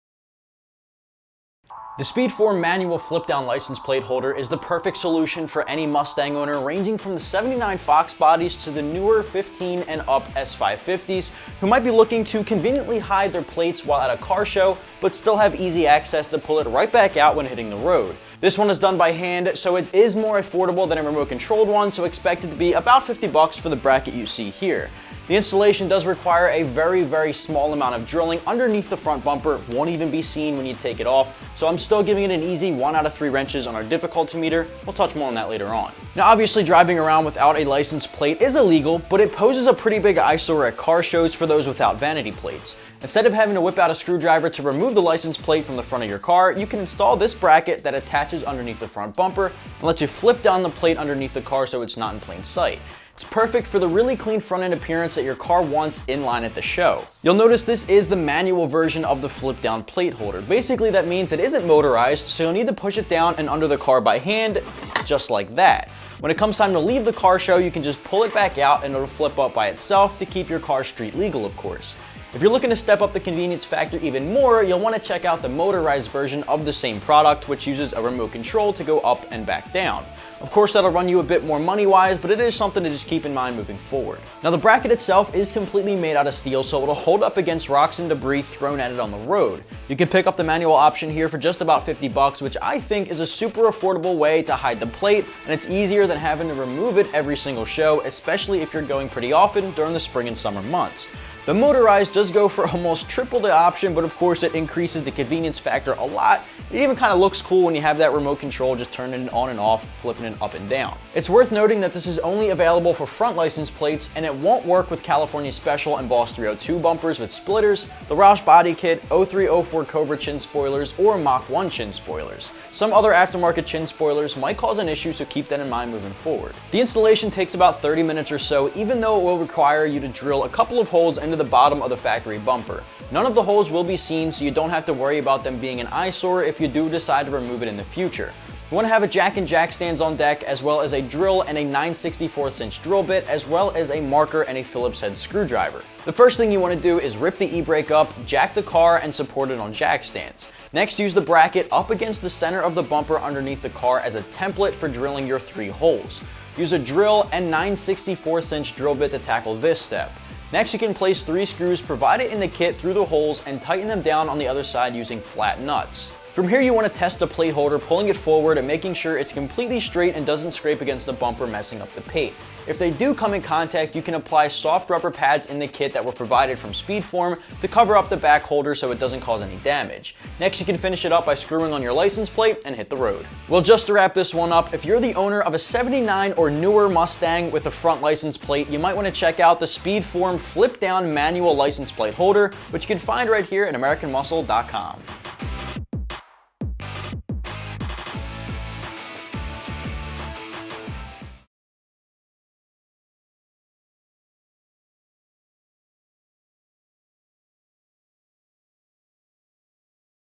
1979-2017 Mustang SpeedForm Flip Down License Plate Holder Review & Install
This SpeedForm Flip Down License Plate Holder allows you to hide your front license plate, and bring it back at any given moment! This License Plate Holder is a universal mounting kit and is designed to fit on all Mustangs.

Item 64501
MPN# 64501

Clean Front End Appearance.With its universal mounting kit, the SpeedForm Flip Down License Plate Holder will fit all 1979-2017 Mustangs. For the show car without vanity plates, this manual Flip Down Holder is a convenient way to temporarily hide the front license plate, while also ensuring the car remains street-legal. Attaching under the front bumper, the SpeedForm Flip Down License Plate Holder enables the front plate to be manually flipped down and hidden underneath the car for a clean front-end appearance. Once back on the road, with the Flip Down License Plate Holder, the front plate is easily reverted back to its upright position.

Quality Construction. The Speedform Flip Down License Plate Holder is manufactured entirely out of steel and will resist against rocks and debris on the road. The included universal mounting kit ensures it is compatible with all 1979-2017 Mustang models, however this manual Flip Down License Plate Holder may not be compatible with certain aftermarket chin spoilers.

Some Drilling Required. To install this Speedform Flip Down License Plate Holder, three holes will need to be drilled – though out of sight under the front bumper. With a jack and jack stands, and a drill with a 9/64” drill bit, the full installation of this Speedform Flip Down License Plate Holder should take approximately 30 minutes to complete.